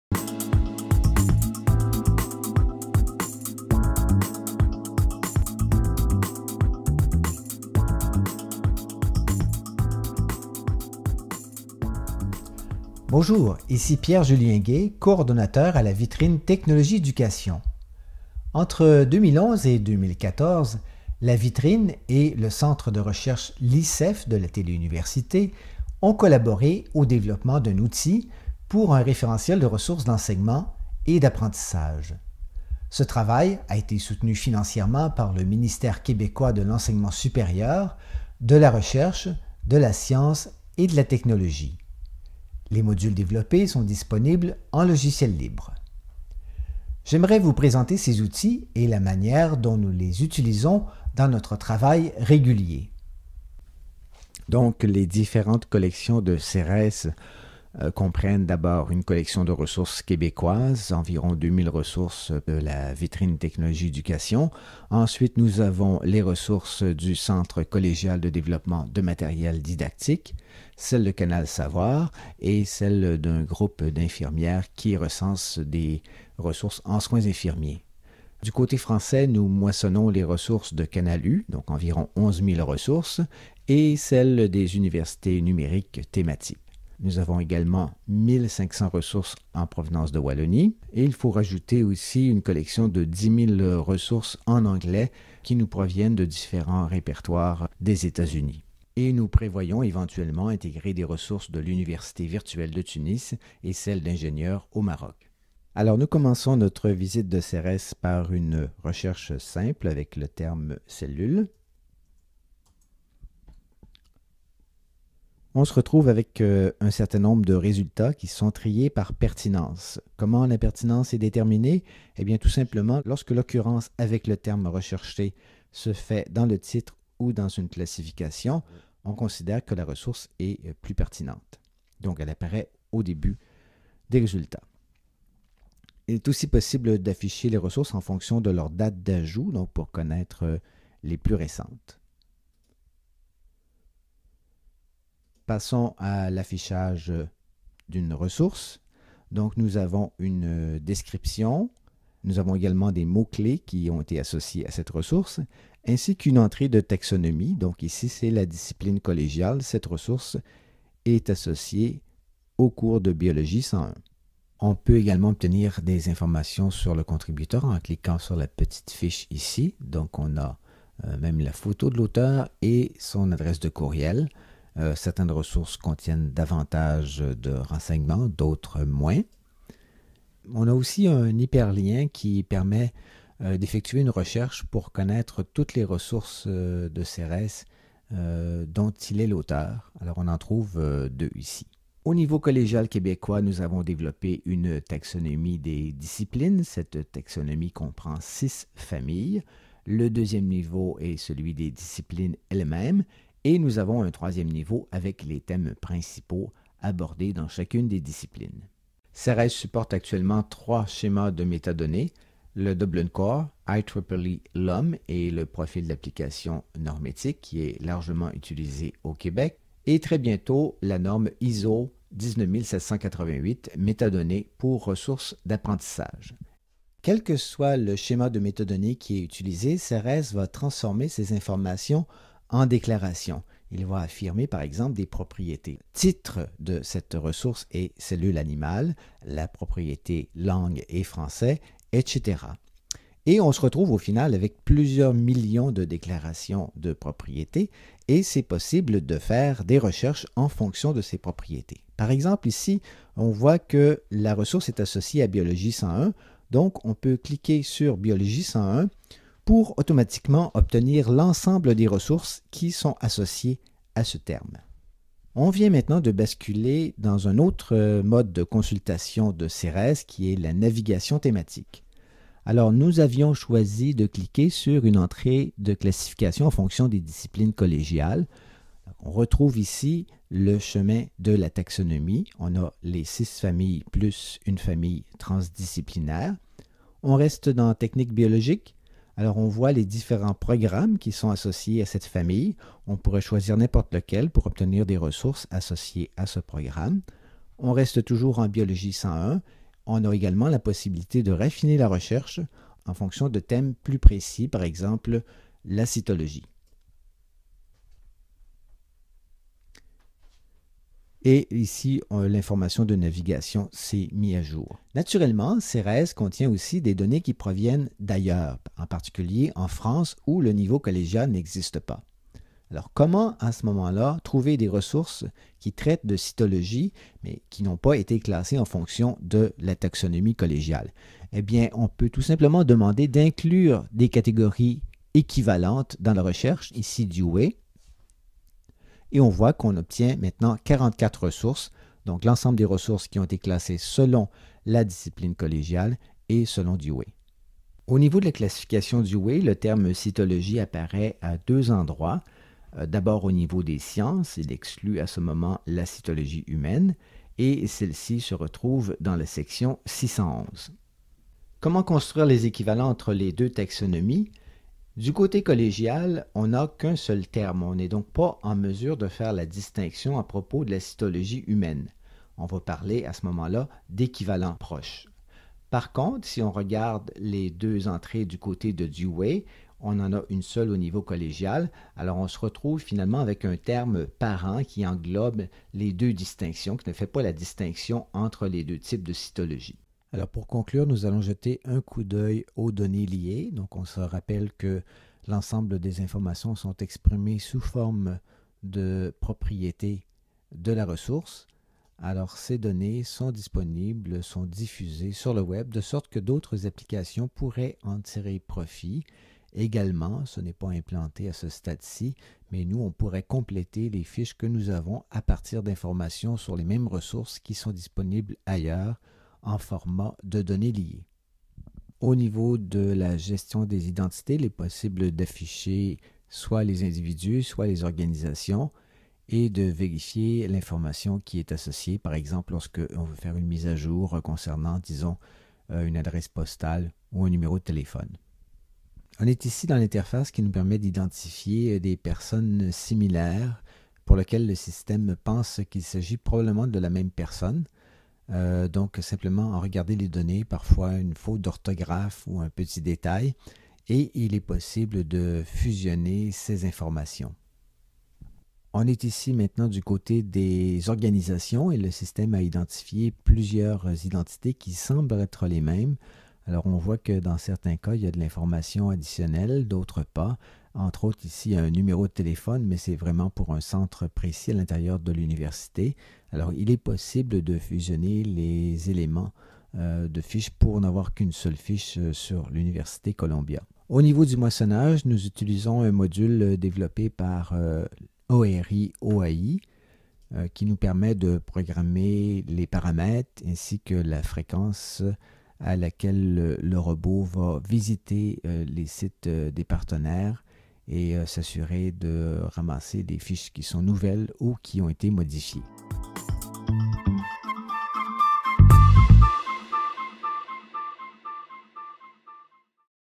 Semaine 3 - CERES un outil de la VTÉ pour rechercher des ressources éducatives
Ceres offre un catalogue collectif de ressources d'enseignement et d'apprentissage rassemblées par divers organismes oeuvrant dans la production et la collecte de ressources éducatives.

La gestion de cet outil de recherche est sous la responsabilité de la Vitrine technologie-éducation.